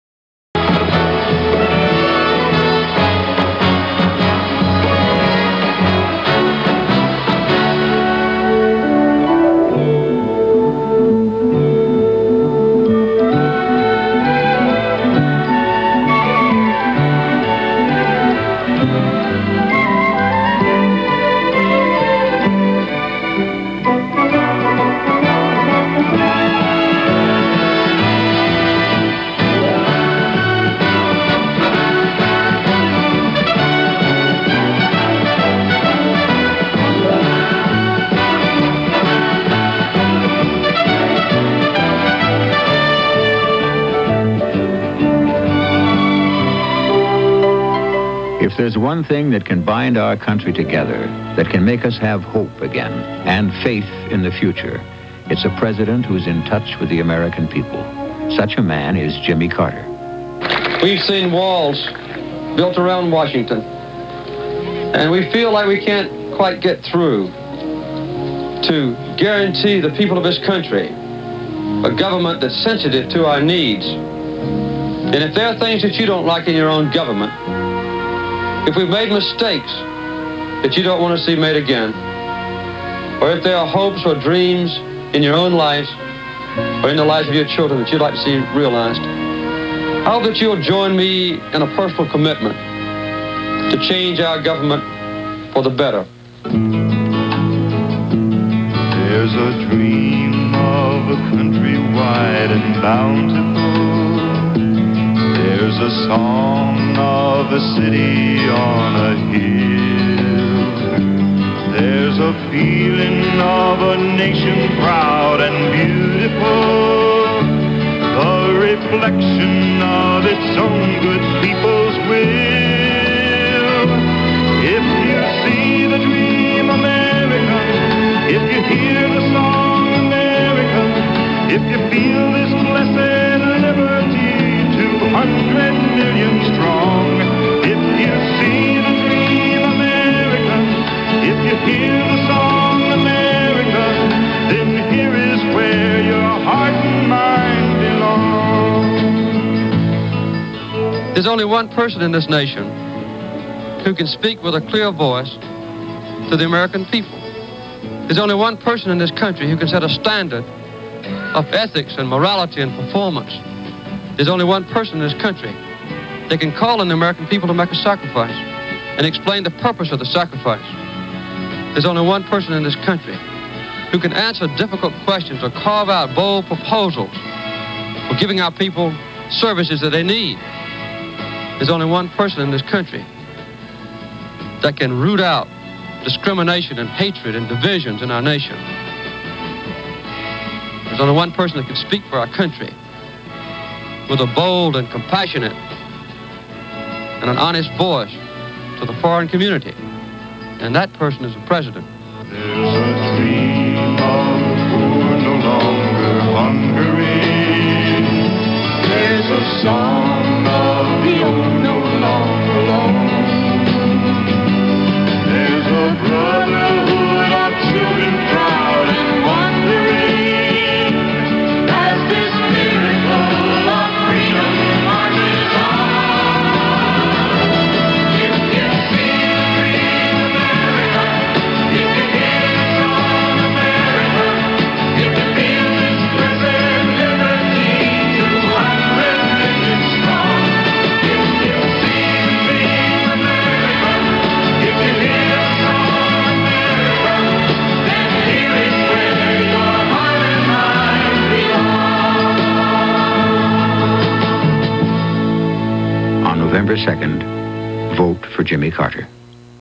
Jimmy Carter [Democrat] 1976 Campaign Ad “[American Song]”
Use of still photo montages and video to capture an optimistic view of American life, complete with music and songs.  Brief  sound bites from candidate inserted at various points.

Creator: Other

Location: Other

Format: 3/4” film; video/mp4

Extent: 4 minutes 20 seconds

Subjects: Leadership, American life